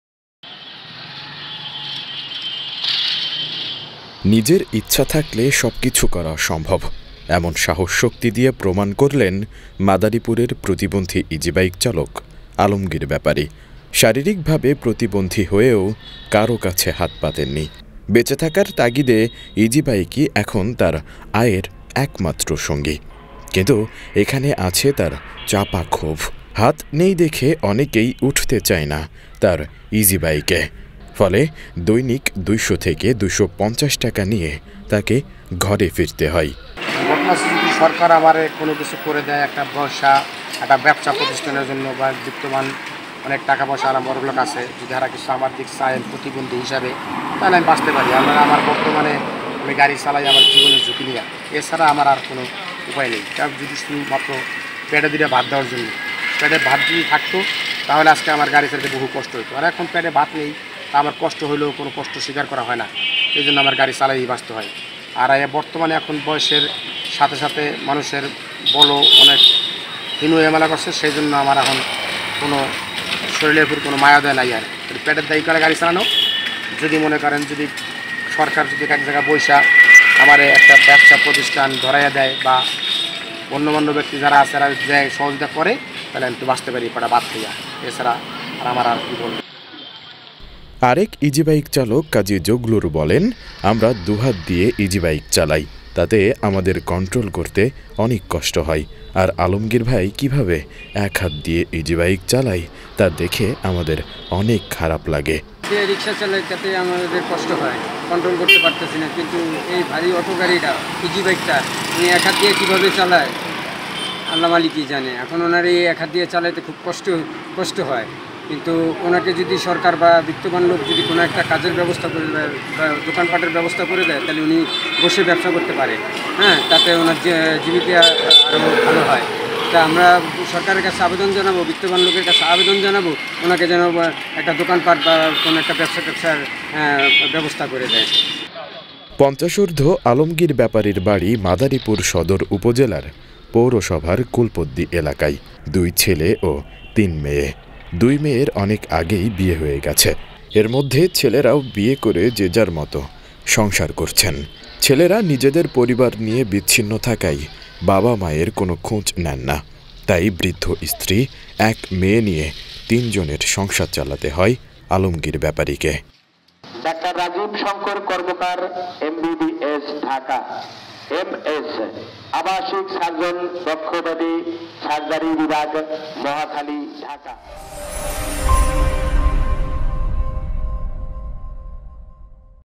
এক হাত দিয়ে আর পারছি না ইজিবাইক চালাতে | madaripur | Dhaka Post
নিজের ইচ্ছা থাকলে সবকিছু করা সম্ভব, এমন সাহস-শক্তি দিয়ে প্রমাণ করলেন মাদারীপুরের প্রতিবন্ধী ইজিবাইক চালক আলমগীর বেপারী।